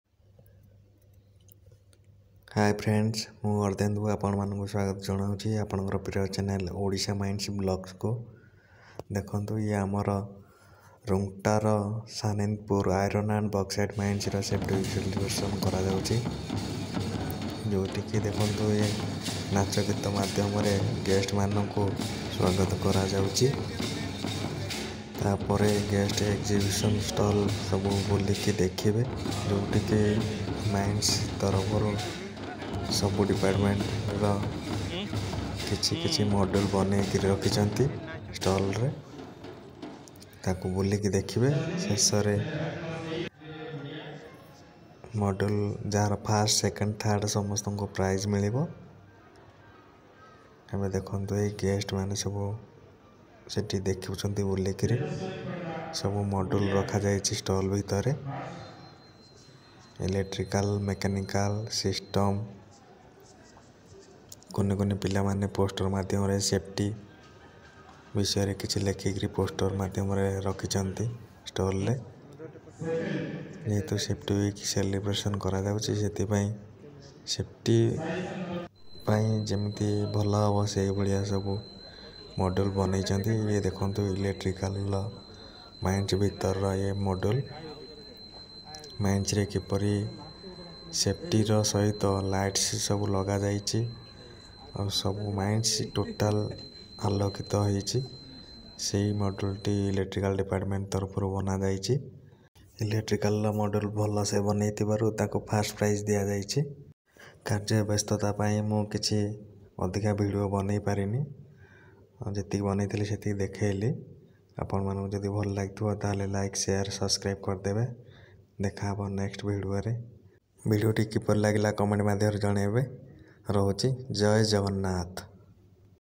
41th Annual Safety Week Celebration 2023 !! Sanindpur Iron & Bauxite Mines !! Rungta Sons Pvt Ltd !!
41th Annual Safety Week Celebration 2023 !! Sanindpur Iron & Bauxite Mines !! Rungta Sons Pvt Ltd !!

41th Safety Week Celebration Sanindpur Iron And Bauxite Mines !!

odisha mines vlogs
odisha mines
mines vlogs
mines